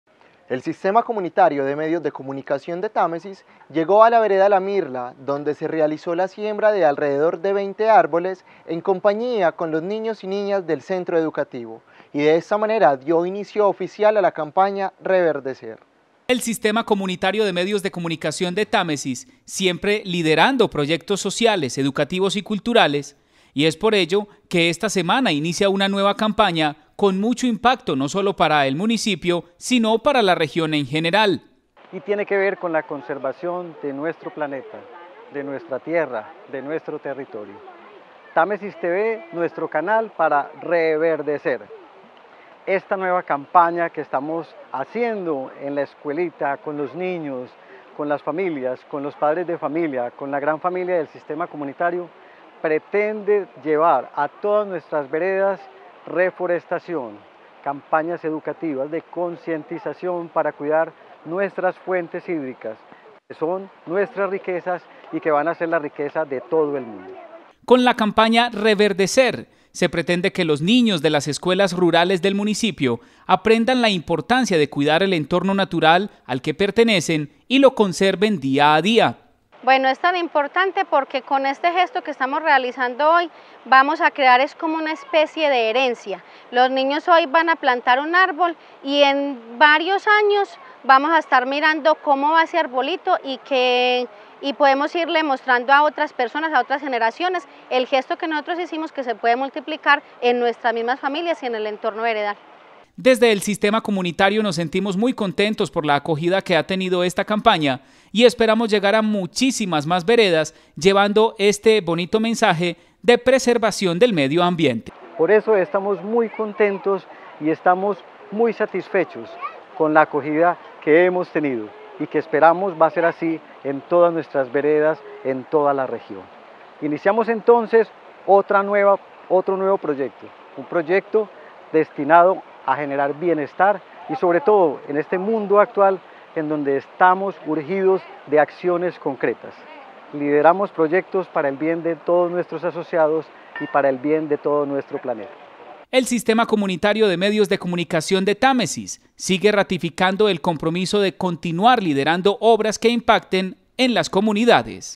Támesis TeVe para Re-verde-ser
El Sistema Comunitario de Medios de Comunicación de Támesis llego C.E.R La Mirla a realizar la siembra de al rededor de veinte árboles en compañía de los niños y niñas del centro educativo y de esta manera se dio inicio oficial a la campaña  Re-verde-ser